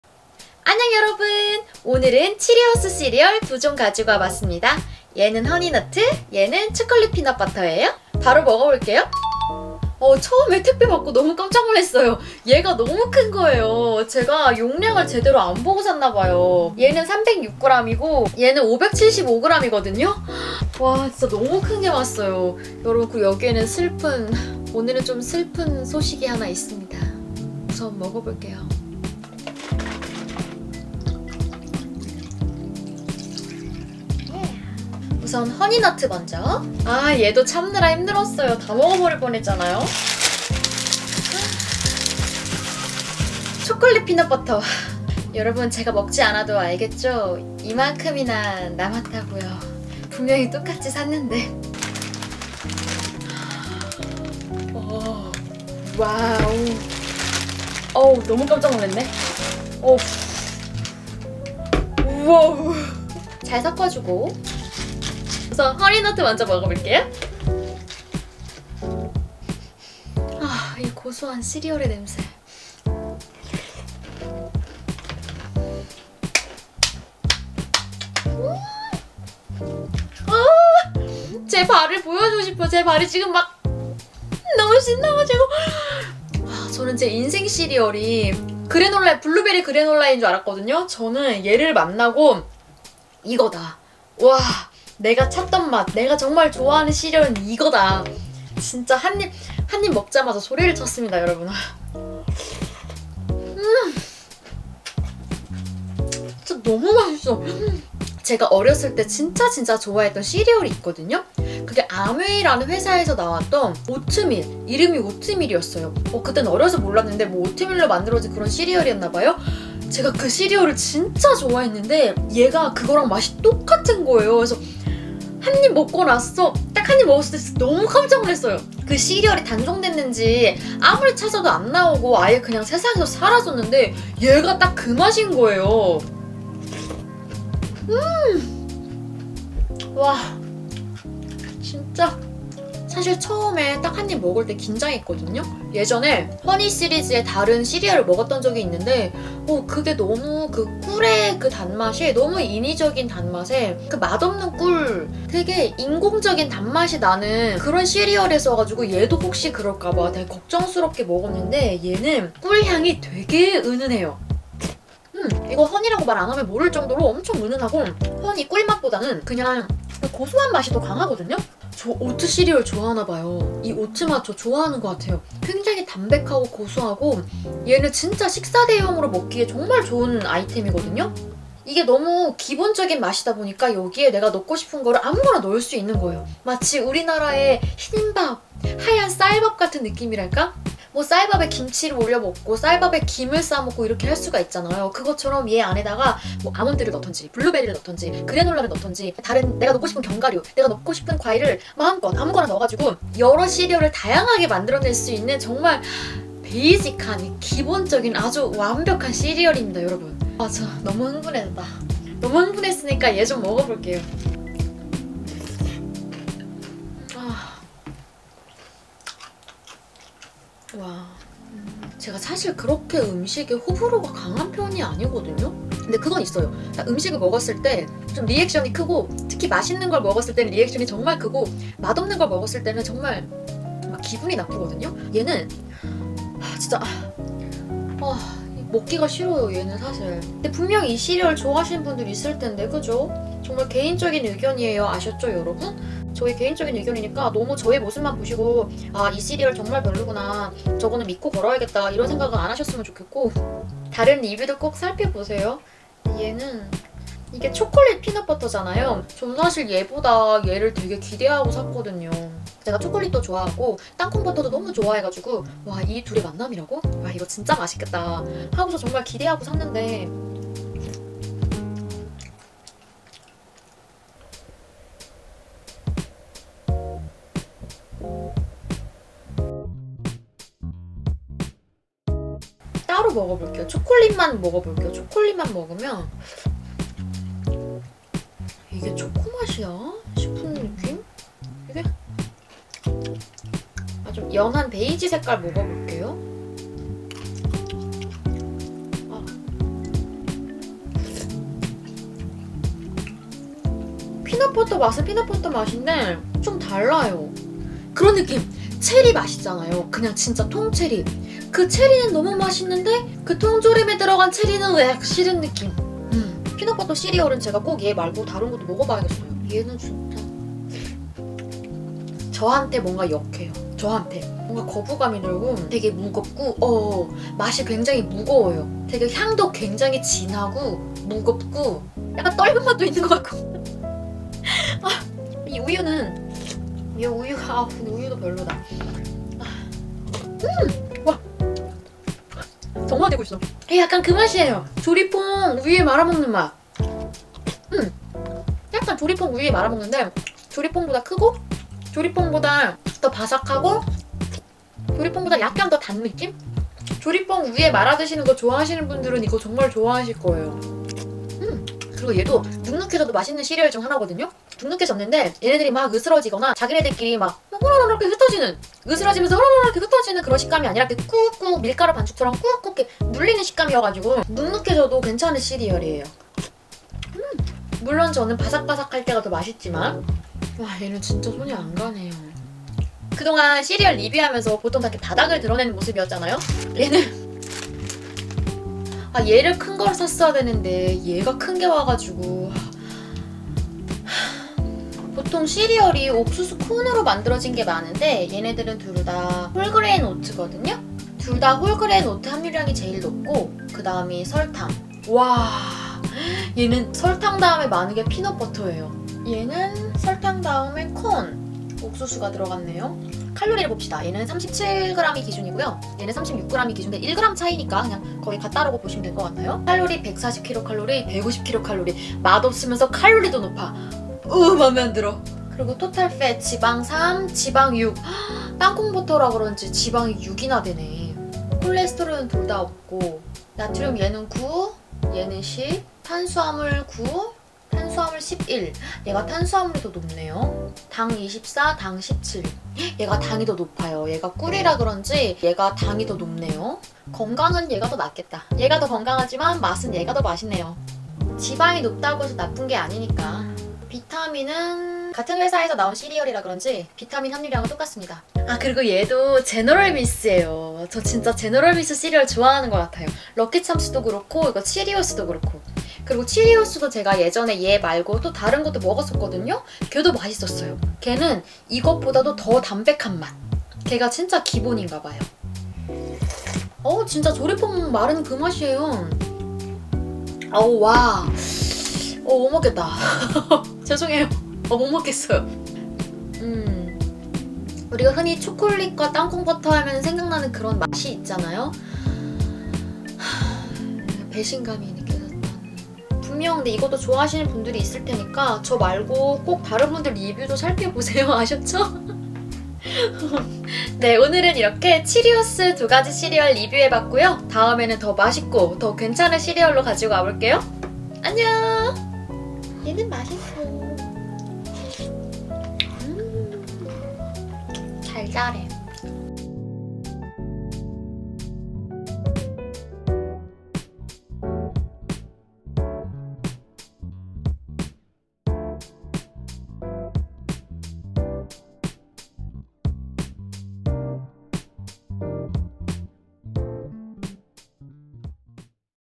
초코 시리얼 & 허니 시리얼은 못 참지! 시리얼 단장의 최애 시리얼 등장?!
안녕 여러분~~!
이번 영상의 시리얼은 제너럴 밀스의 치리오스 2종 입니다!
허니 너트 & 초콜릿 앤 피넛버터